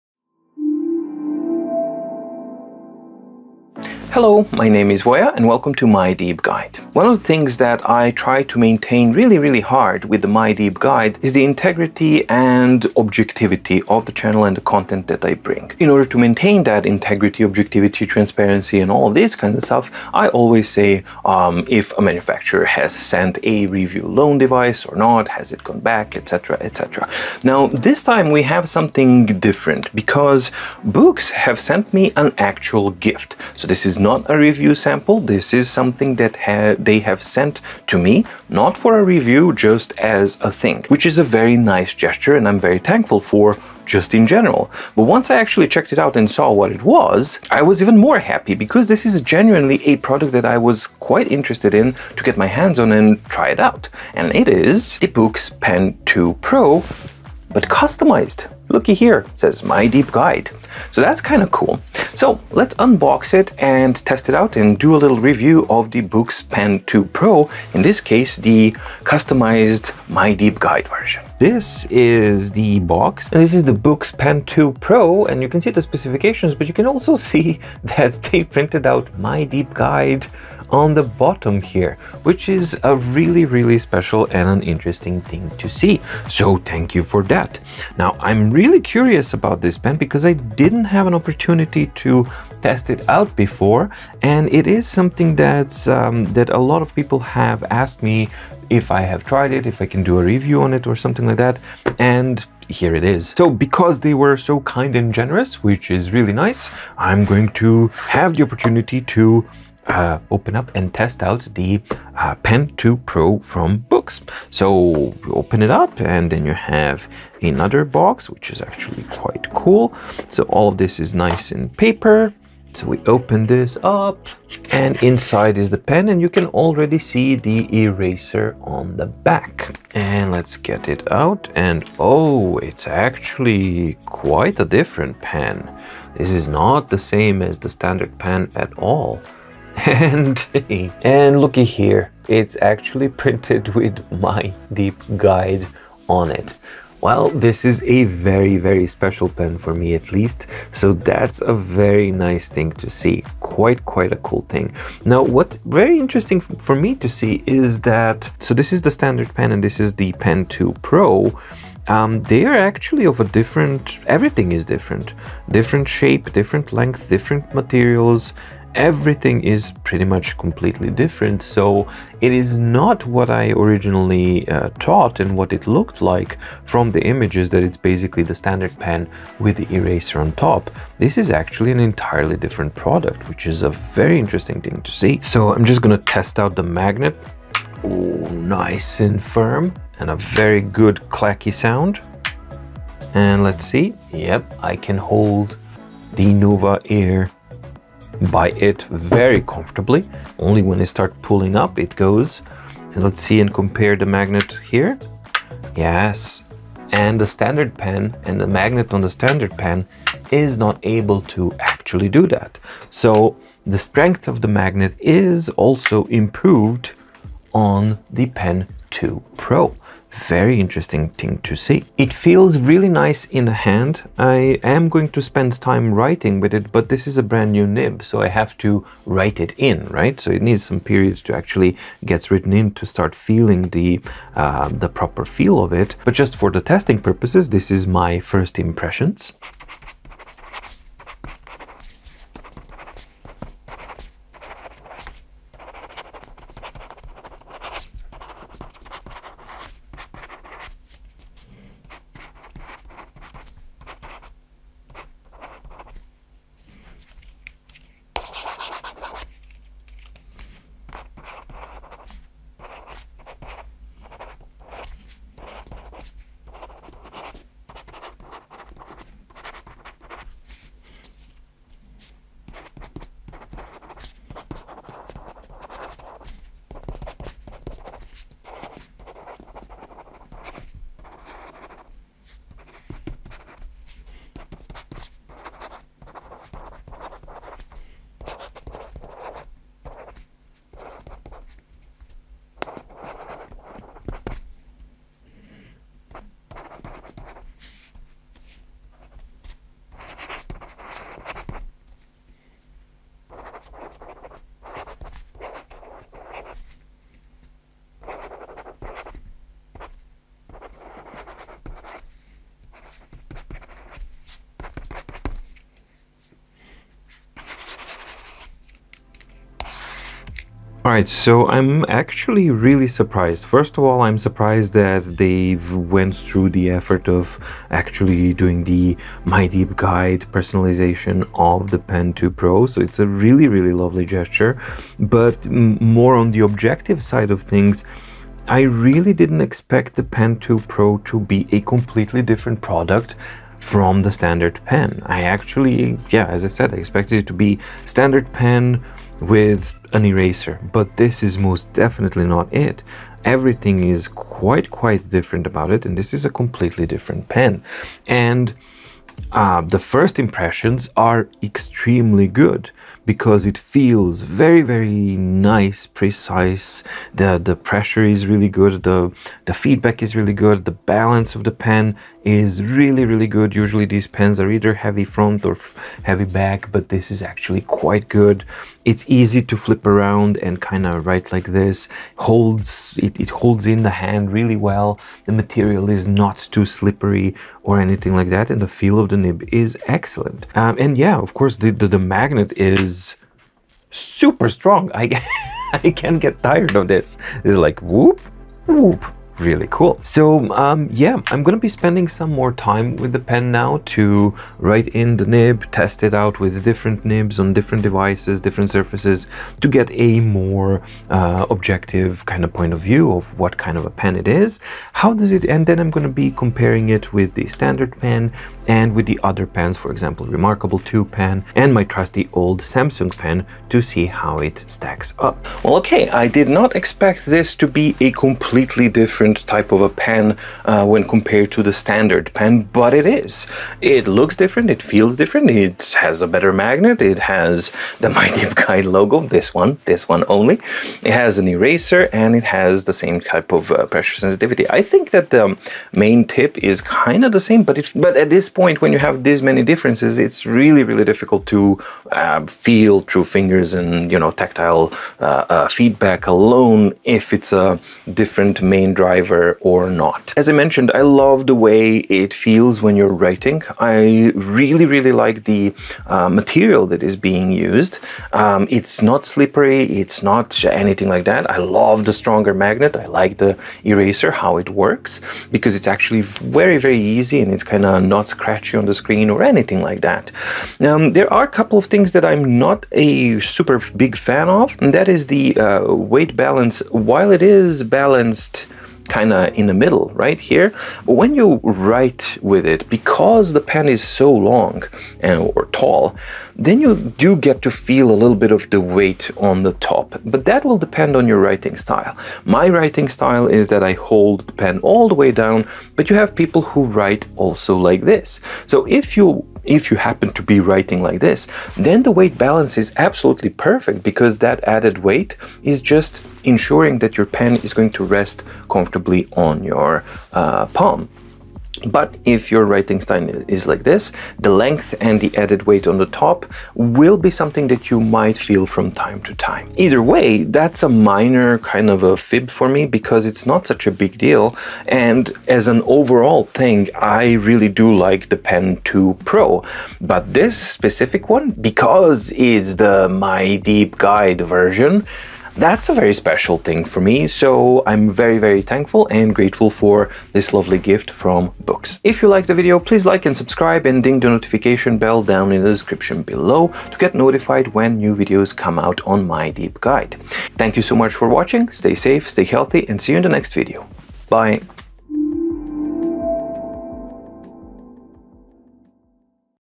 Boox Pen2Pro Pen: Unboxing And First Impressions
00:00 - Intro
01:11 - Unboxing
02:21 - The Pen
03:59 - Writing Example
07:05 - Conclusion

I received a very thoughtful gift from Boox, so let's open it up and check it out :)